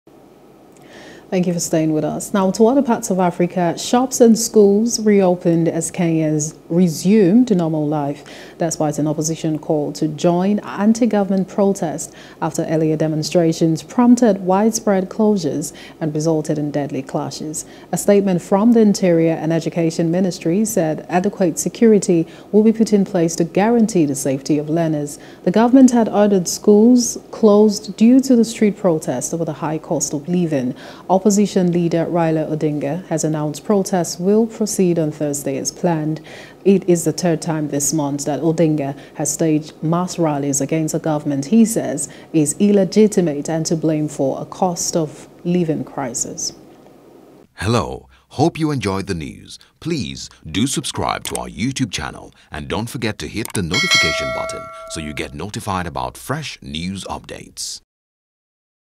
Kenya: Shops, Schools Reopen Despite Protest Call
Shops and schools reopened as Kenyans resumed normal life, despite an opposition call to join anti-government protests after earlier demonstrations prompted widespread closures and resulted in deadly clashes.